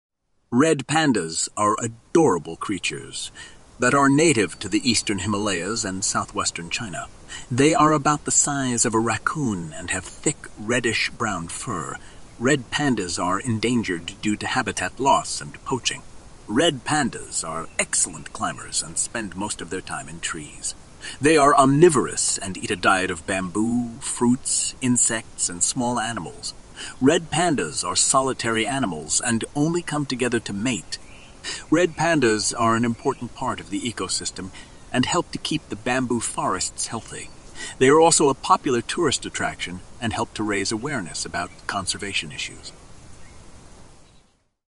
The Red Panda The Adorable Animal That Looks Like a Raccoon and a Bear
Red pandas are adorable creatures that are native to the Eastern Himalayas and southwestern China. They are about the size of a raccoon and have thick, reddish-brown fur. Red pandas are endangered due to habitat loss and poaching.

Red pandas are excellent climbers and spend most of their time in trees. They are omnivorous and eat a diet of bamboo, fruits, insects, and small animals. Red pandas are solitary animals and only come together to mate.

Red pandas are an important part of the ecosystem and help to keep the bamboo forests healthy. They are also a popular tourist attraction and help to raise awareness about conservation issues.